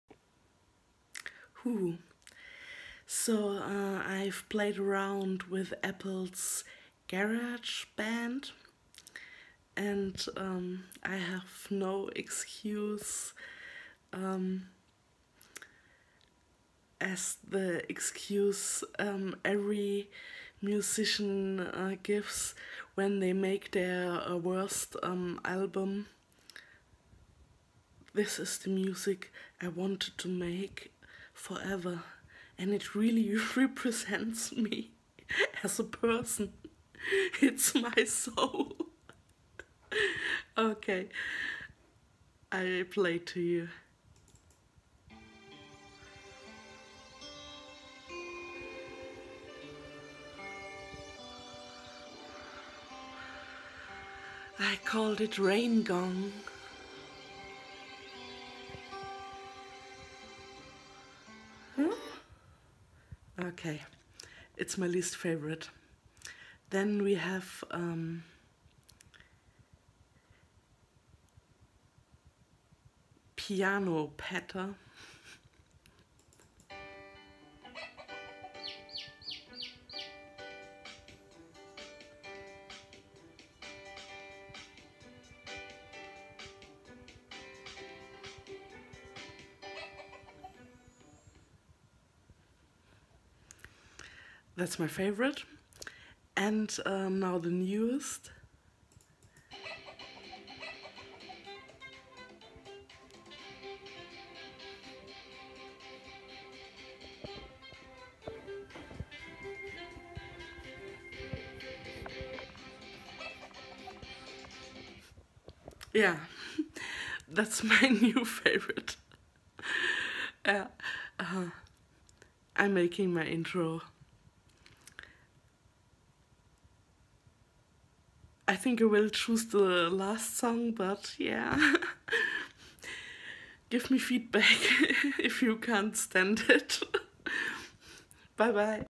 VLog 9: Intro Jingles made with GarageBand
Played around with the Apple App Garage Band. Love it! I need an intro jingle ... pls feedback